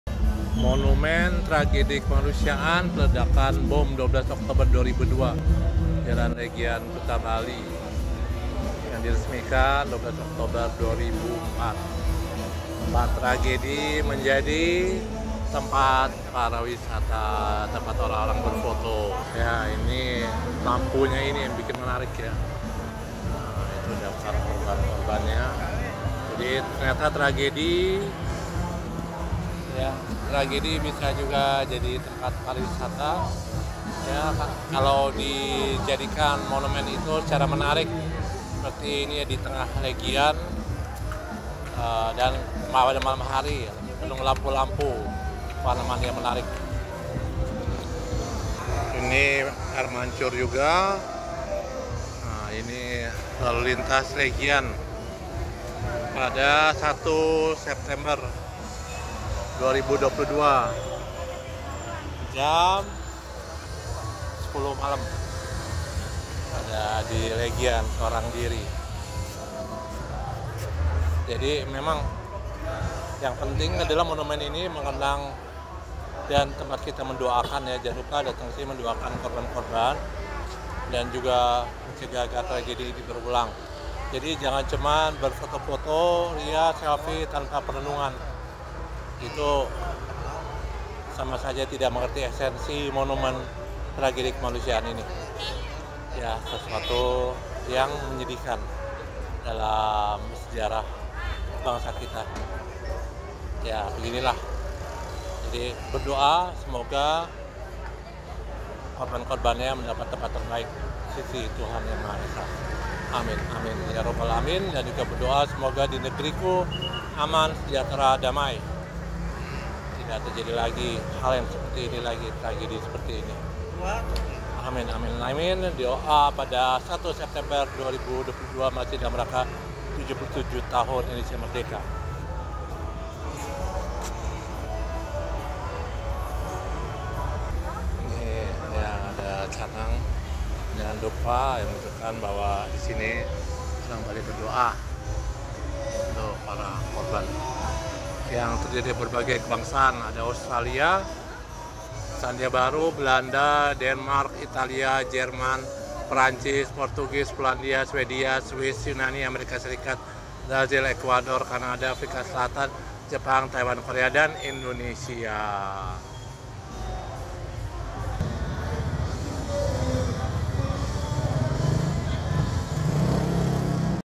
Monumen "Selfie"
Ketika Monumen Tragedi Kemanusiaan menjadi spot foto dan selfie favorit di Legian...

*Monumen Tragedi Bom Bali 2002 pada 1 September 2022 jam 10 malam